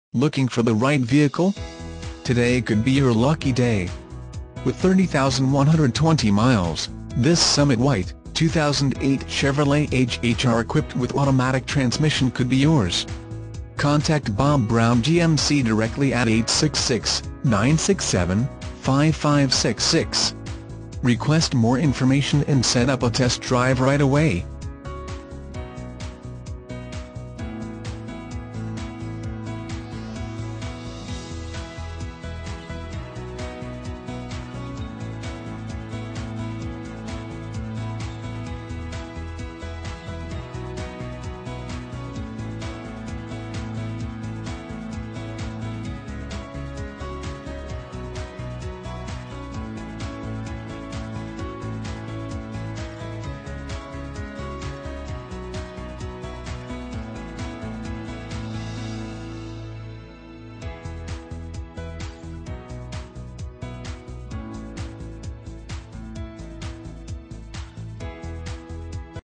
2008 Chevrolet HHR - Sport Utility Ankeny Iowa 054487H
2008 Chevrolet HHR

Bob Brown GMC
1405 SE Oralabor

Looking for the right vehicle? Today could be your lucky day. With 30120 miles, this Summit White, 2008 Chevrolet HHR equipped with Automatic transmission could be yours. Request more information and set up a test drive right away.